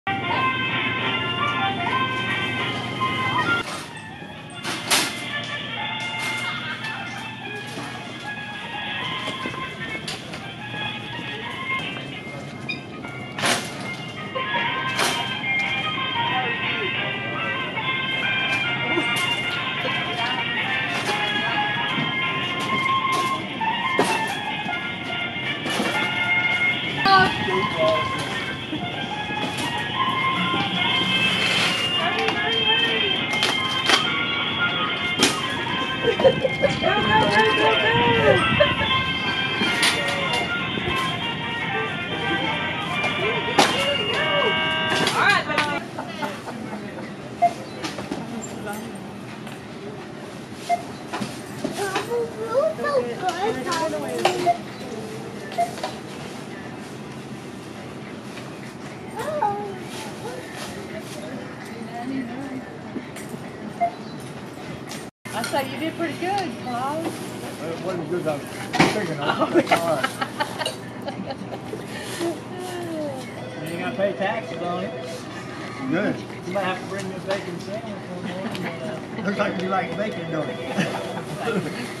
Claude Gulley Wins Brookshire Bros. "Price is Right Shopping Spree'
Claude Gulley of Woodville won a one minute dash through Brookshire Bros in Woodville, Texas in the "Price is Right Shopping Spree.' Gulley picked up $398.20 worth of merchandise during his dash through the store.